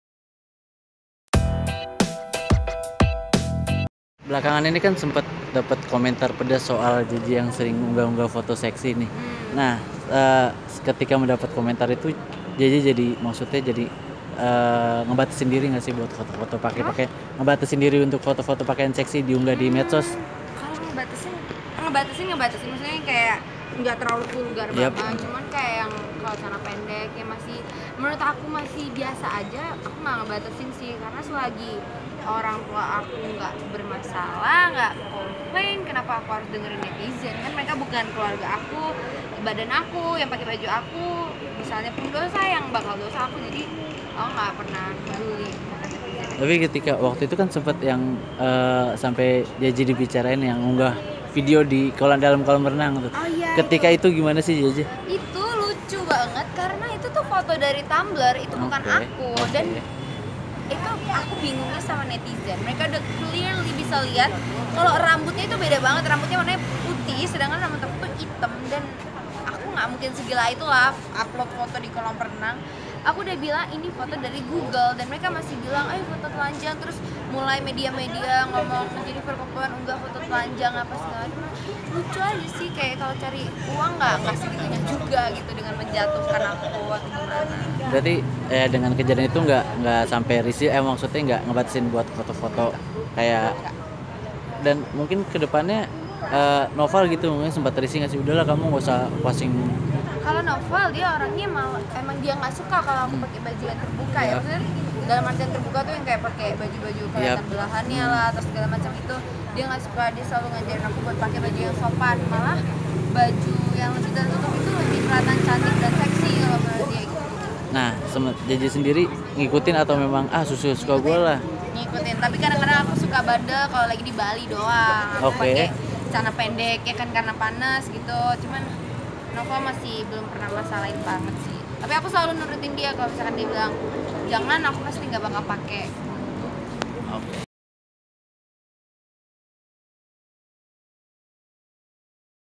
Jennifer Coppen Bantah Video Tanpa Busana di Kolam Renang
Bahkan, beberapa waktu lalu, Jennifer Coppen sempat mengunggah sebuah video tanpa busana saat sedang berenang, yang kemudian dihapus olehnya.

Unggahan Jennifer Coppen itu memancing komentar warganet atau netizen, yang langsung menyerangnya lewat cacian dan makian dalam kolom komennya.

"Oh video itu... oh itu lucu banget karena video dari thumbler... Itu bukan aku. Kemudian ya aku unggah di media sosial aku ya sebagai bantahan," kata Jennifer Coppen kepada Warta Kota, ketika ditemui di gedung Trans TV, Jalan Kapten Tendean, Mampang Prapatan, Jakarta Selatan, Senin (18/2/2019). (Arie Puji Waluyo/ARI).